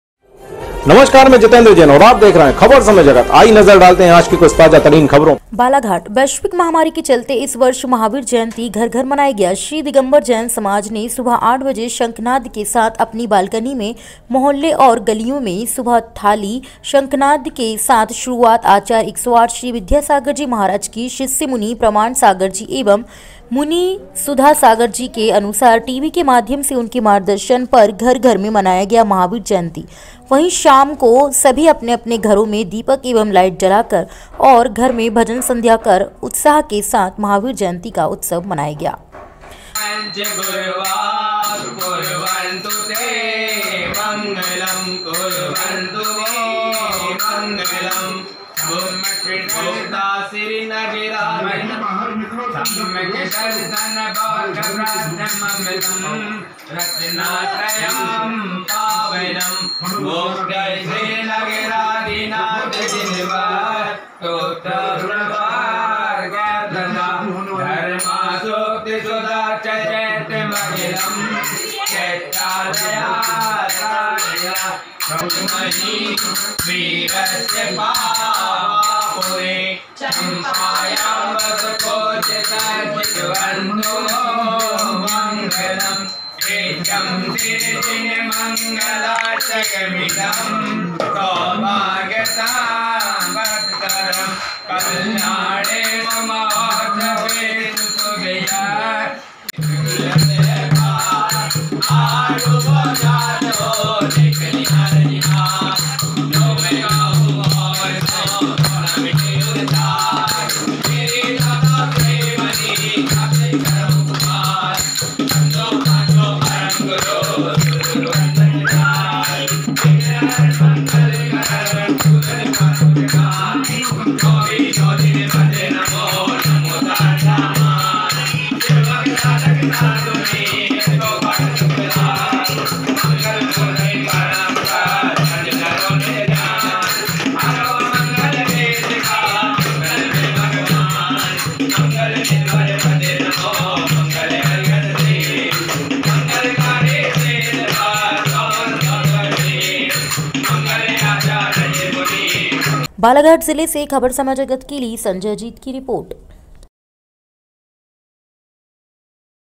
बालाघाट जिले में 25 को घर घर में मनाया गया महावीर जयंती।
बालाघाट जिले से संजय अजित की रिपोर्ट